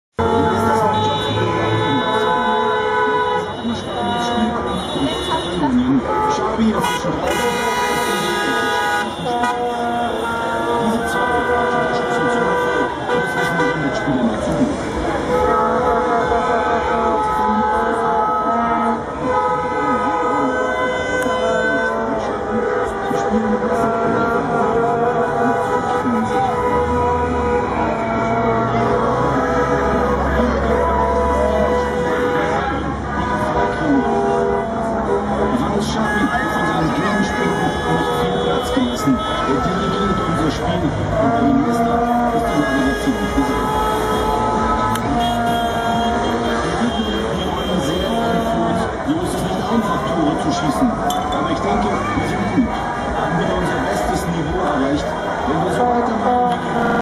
Lüneburg - Jubelfeiern WM 2010 - HD Qualität
Jubelfeiern in Lüneburg nach dem 4:0 Erfolg gegen Argentinien im Viertelfinale
der WM 2010 am 03.Juli 2010.